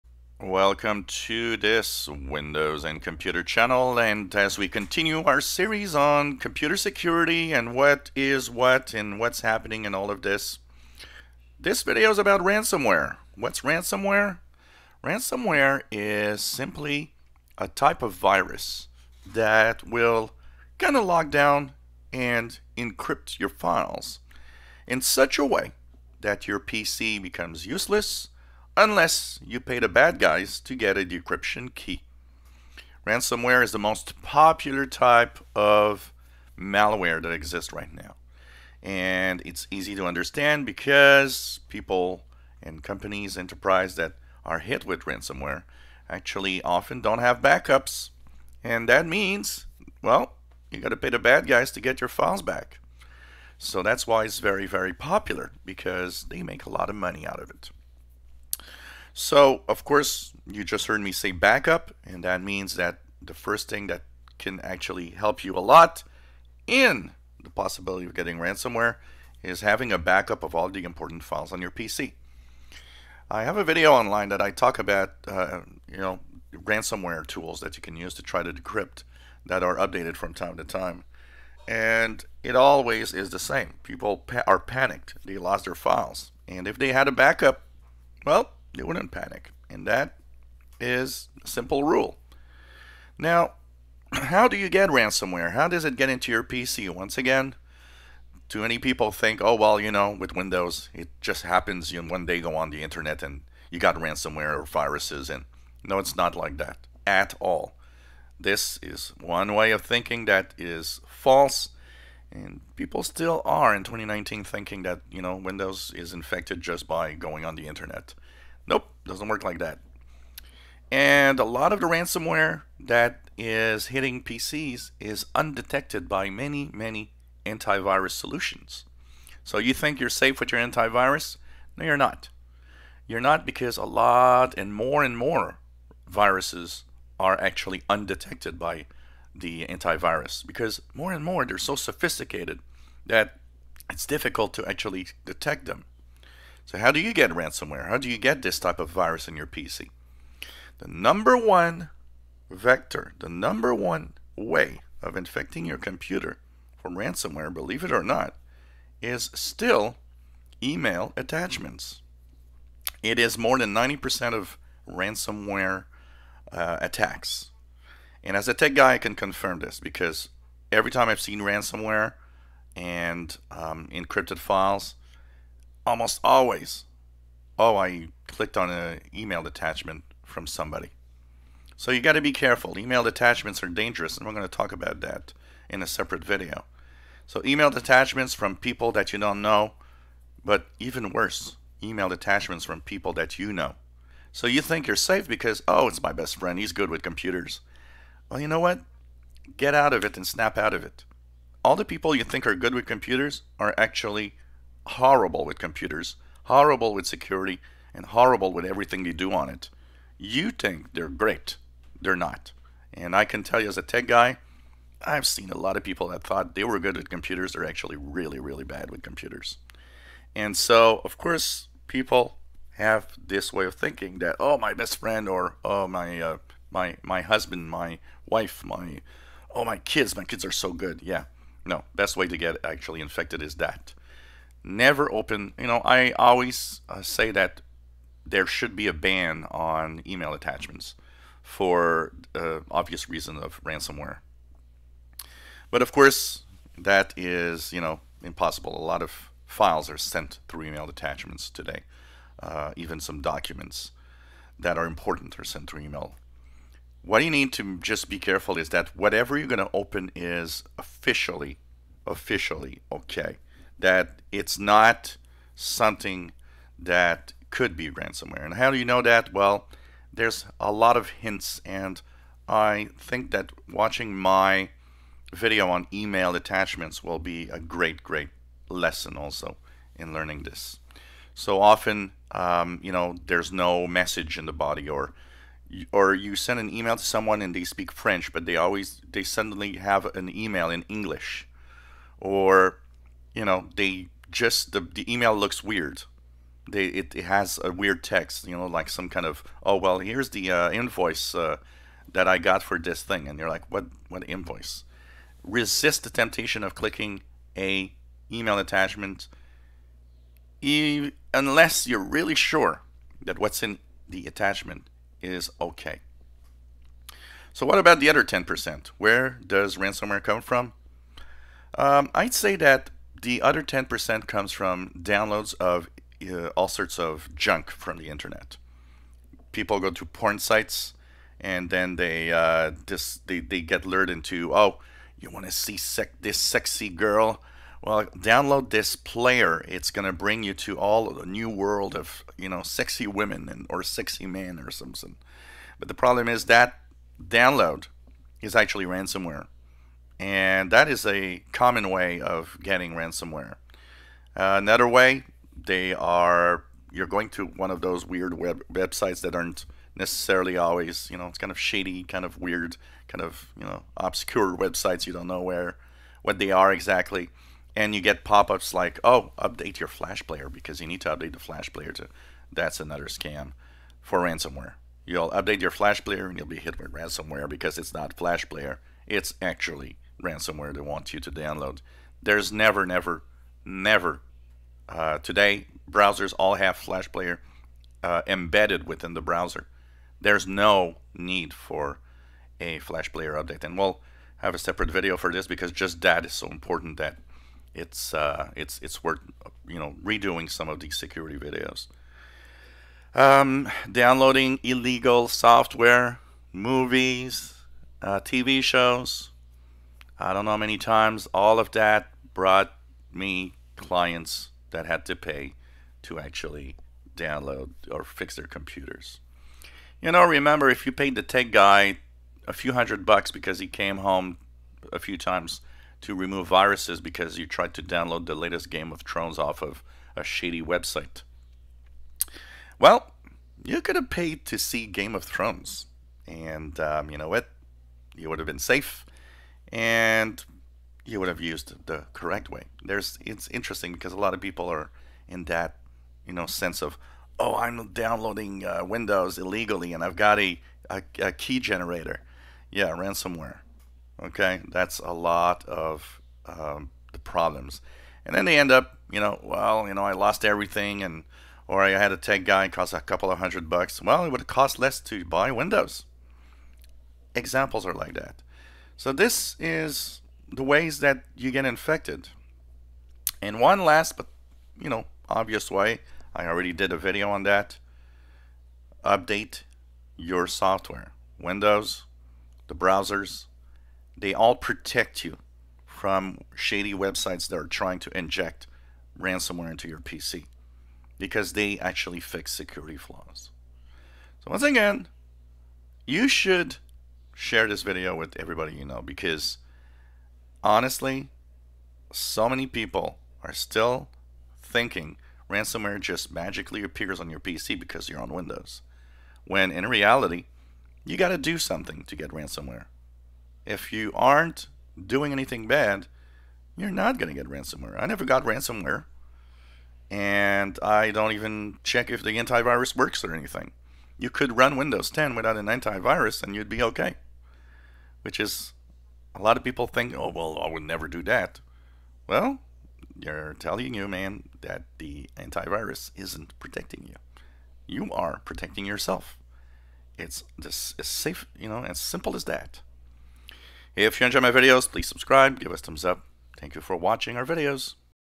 IMPORTANT SECURITY What is Ransomware and How do you get infected
Ransomware is the most dominant Virus type that exists today